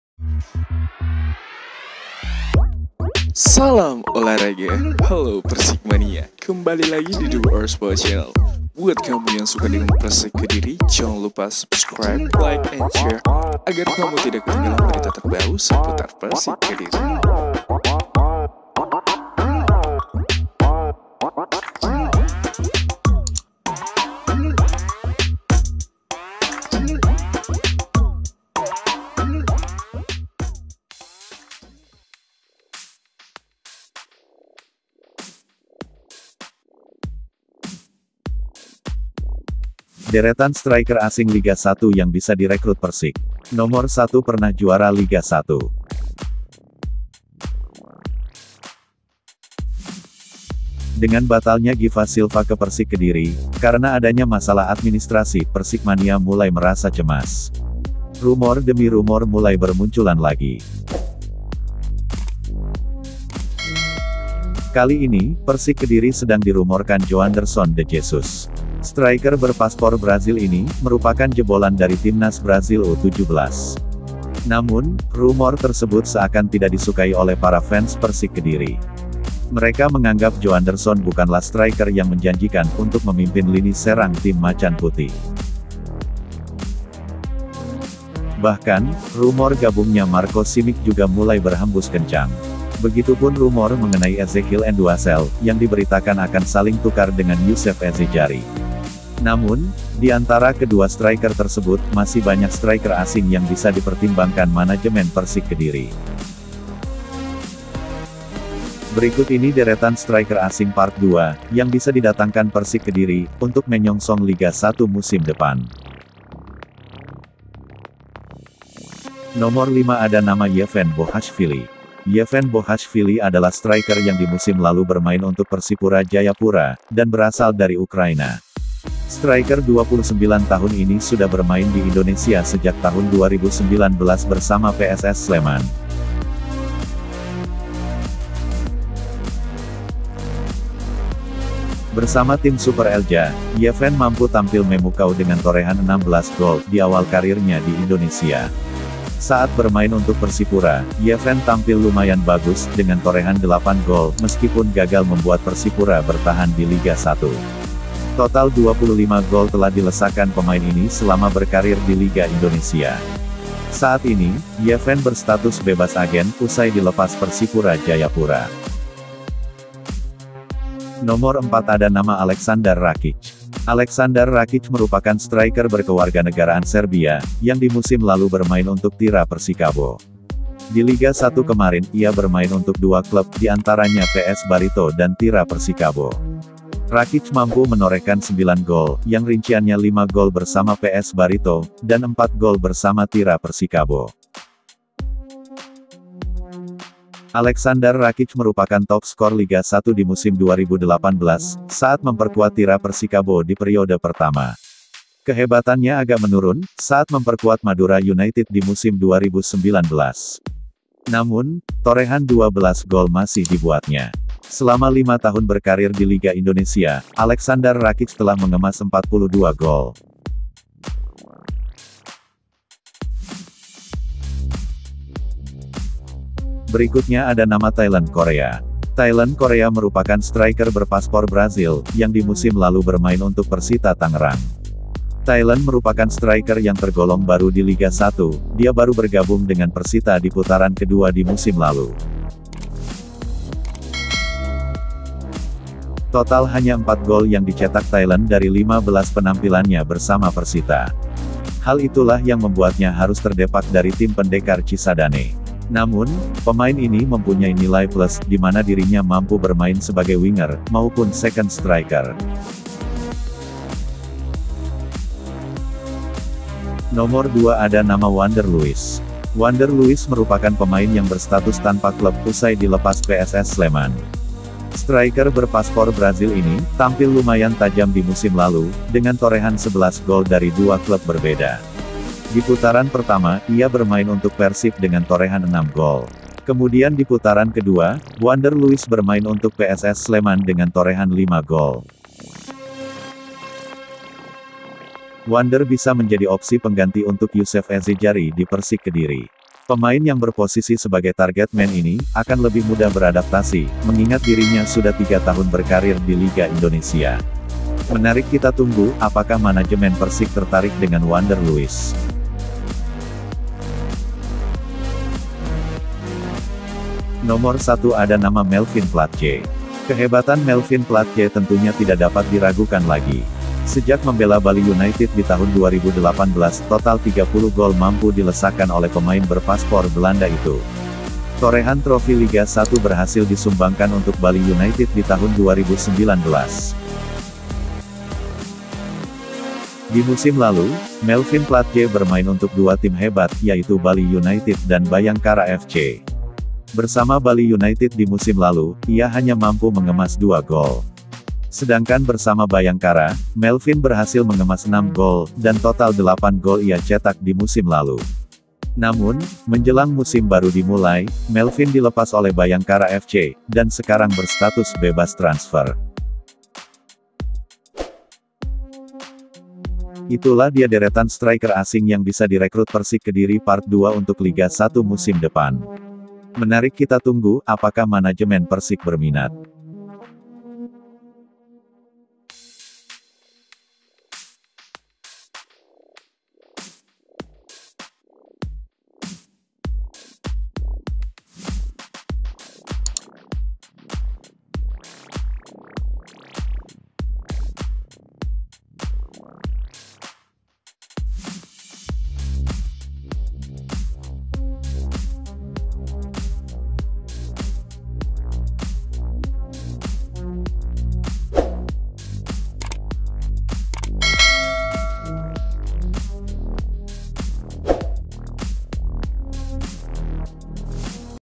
KABAR PERSIK : GACORR!! 👍 DERETAN PEMAIN ASING LIGA 1 PART 2 YANG BISA DIREKRUT PERSIK KEDIRI!!
SUMBER : persik official
Pictures From Instagram : Persik Official, Giva Silva, Joanderson, Marko Simic, Ezechiel N'douassel, Yevhen Bokhashvili, Aleksandar Rakic, Taylon Correa, Wander Luiz, Melvin Platje